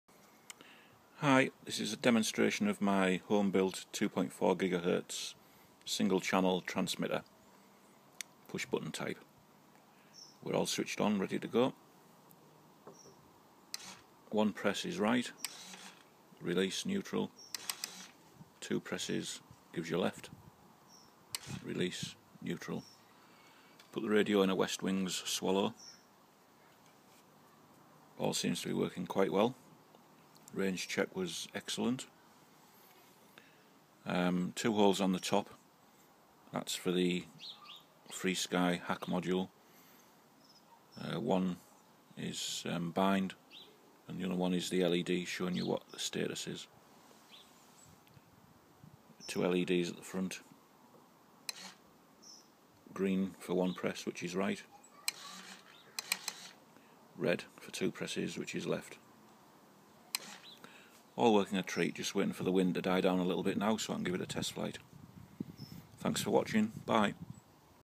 Single channel 2.4 ghz home built transmitter demo.
Single channel 2.4 ghz home built transmitter demo. Radio is installed in a westwings swallow.

follow me at  priory-electronic-supplies on eBay for transmitter and electronic parts